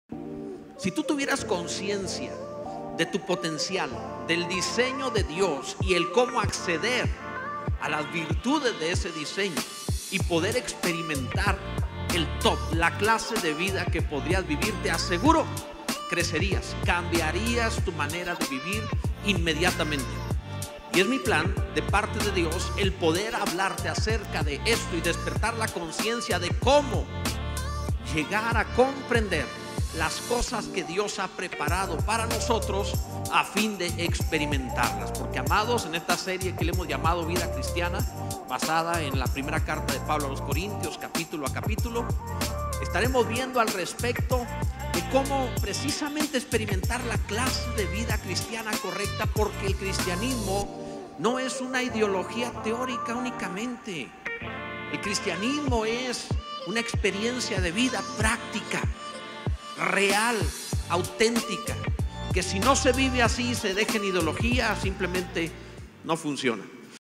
¡Con tu potencial crecerías!
Si tuvieramos conciencia del potencial, del diseño de Dios y de como acceder a las virtudes de ese diseño...
Creceríamos, cambiariamos nuestra manera de vivir inmediatamente.